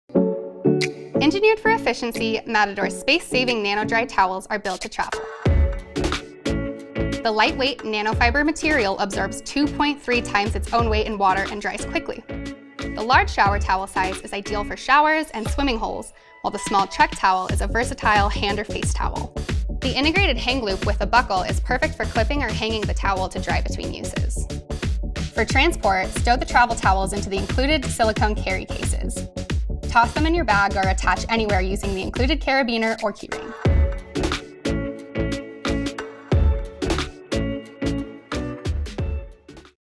Matador NanoDry Towel Demo
Designed for lightweight, efficient travel. The nanofiber material absorbs 2.3x its own weight in water and dries quickly. An included silicone case with a carabiner makes it easy to attach the towel to the outside of your pack when not in use.